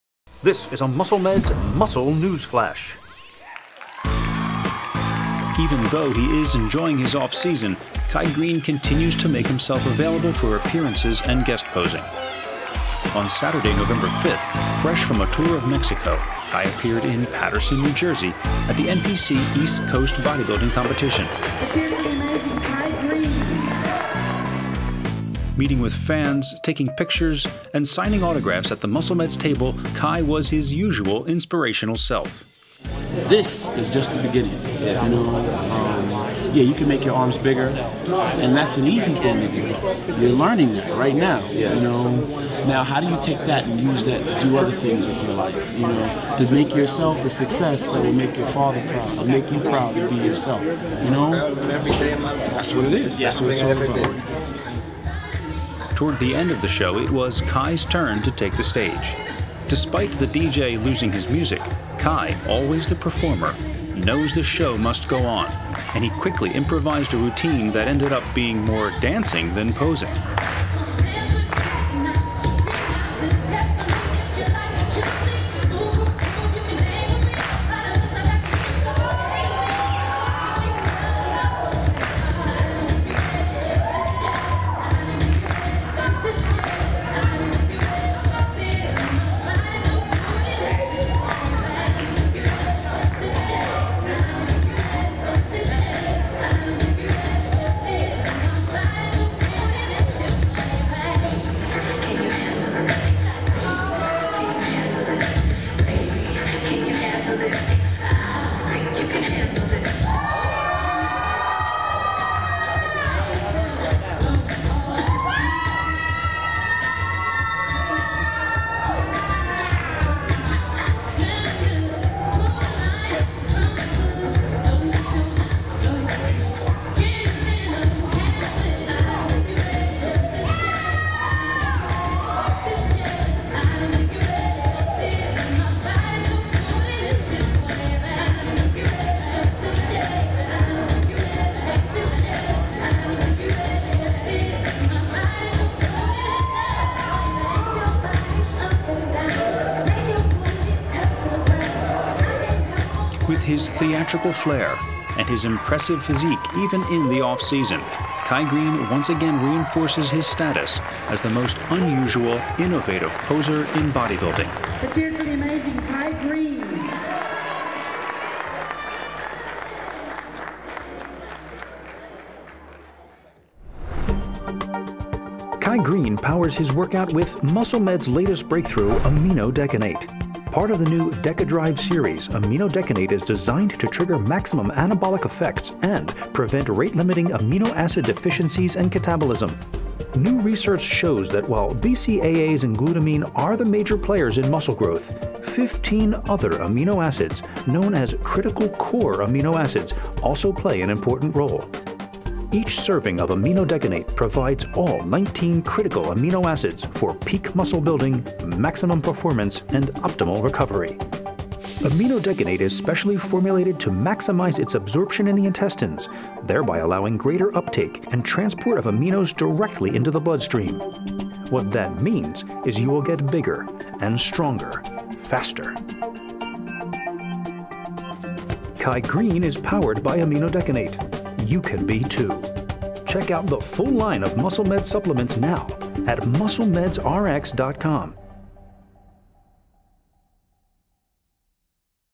Kai Greene guest posing and personal appearance 11_5_11
Kai Greene guest posing at the NPC East Coast bodybuilding competition in Paterson, New Jersey. Promoter: Maz Ali of Body Worx Gym.